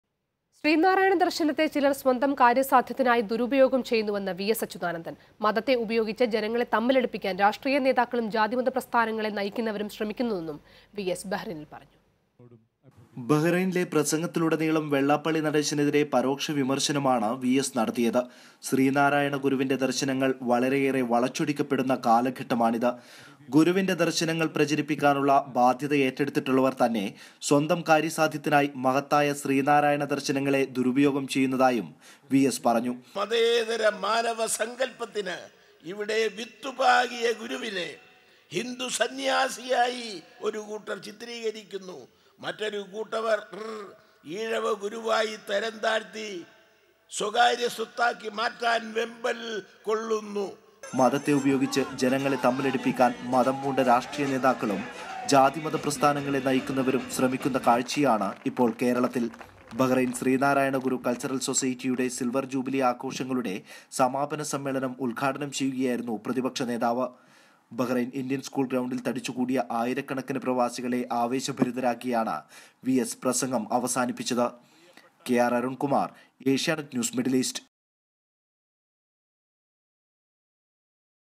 VS Achuthanandan in Bahrain | Asianet Gulf News
VS Achuthanandan to Participate In Bahrain Sree Narayana cultural Society silver jubilee Celebrations VS Achuthanandan in Bahrain
വി എസ് ബഹറിനില്‍ മതത്തിന്റെ പേരില്‍ ജനങ്ങളെ തമ്മിലടിപ്പിക്കു ന്നു